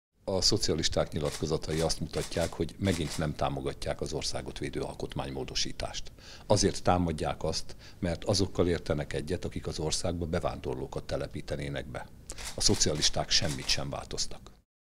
A szocialisták megint nem támogatják az országot védő alkotmánymódosítást
A szocialisták azokkal értenek egyet, akik az országban bevándorlókat engednének be. A szocialisták semmit sem változtak – hangsúlyozta Halász János csütörtökön portálunknak.